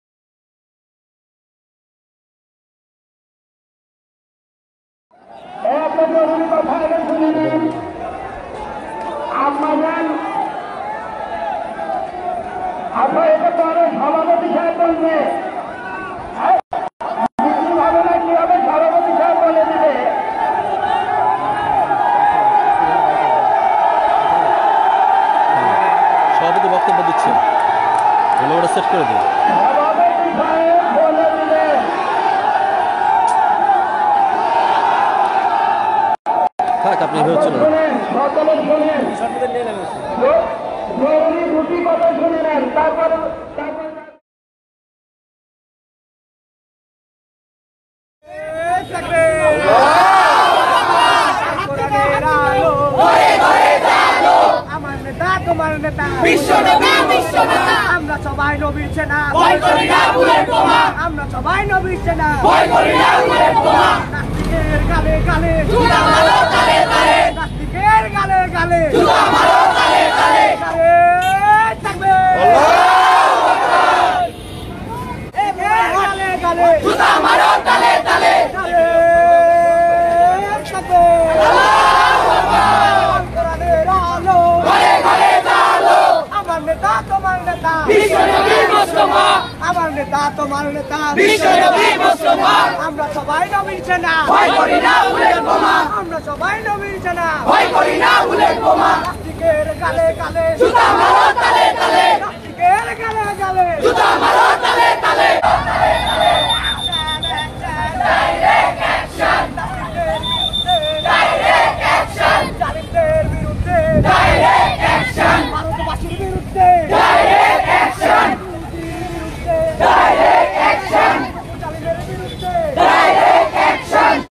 ॥প্রতিবাদে বিক্ষোভ॥
গত ২০২২-০৬-১০ তারিখে-

নাটোর কেন্দ্রীয় মসজিদের সামনে বিক্ষোভ সমাবেশকে কেন্দ্র করে এক জন সমুদ্রে পরিনত হয়।

মহানবী(স:)কে নিয়ে ক, টু, ক্তির প্রতিবাদে বিক্ষোভ
আয়োজনে শেরে বাংলা উচ্চ বিদ্যালয়ের শিক্ষার্থীরা